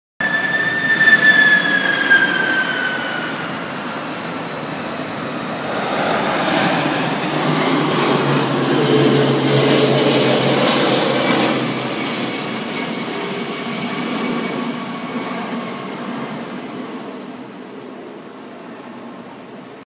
Somma Lombardo: airbus 330 Qatar in atterraggio 17R Malpensa
Un A330 della Qatar sta atterrando sulla pista 17 destra causa vento da sud e pista 17L chiusa per lavori.
Ecco il Kay Tak sommese